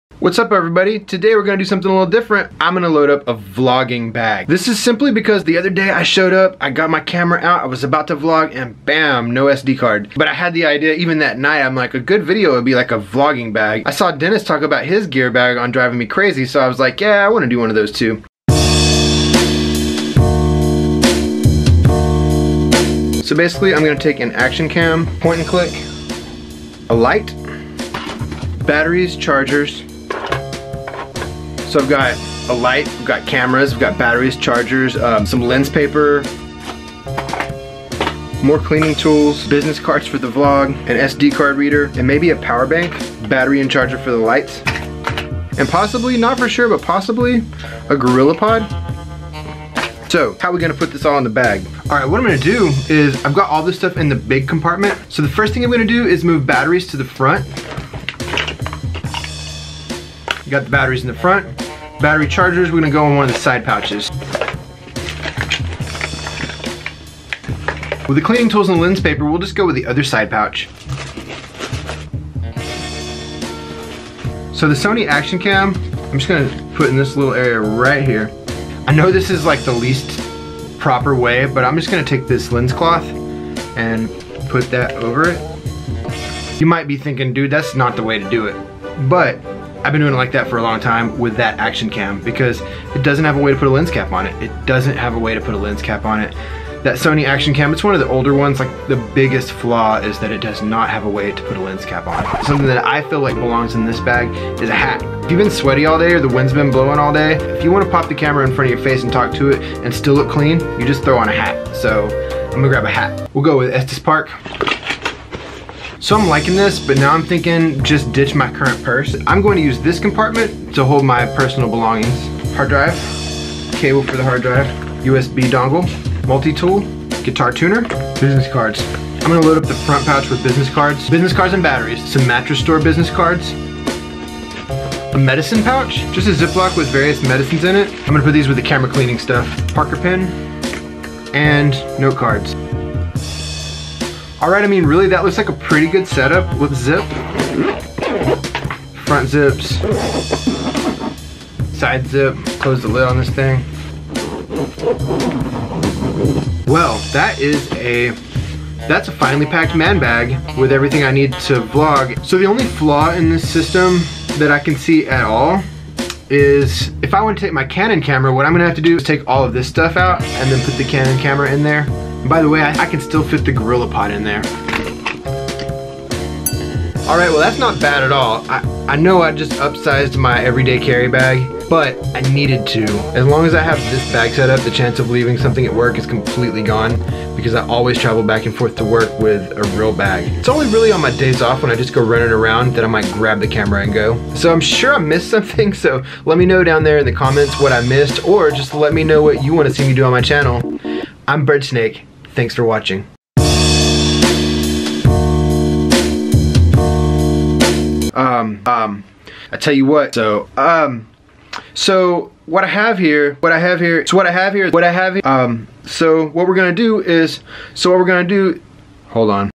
Vlogging Tips: VLOG GEAR BAG + EDC MAN BAG
I load up my vlogging gear bag and combine it with my edc man bag.

EDC is every day carry if you didn't know, so I guess this is like a vloggers gear everyday carry bag. I saw a few other guys doing "what's in my gear bag" videos and just jumped on. I always enjoy figuring out how to pack my gear bags... it's fun for me, so it's extra fun when it's my vlogging gear bag combined with my man bag. This isn't about what camera do I use or how to vlog, but more of just a packing video. What's in your vlog gear bag?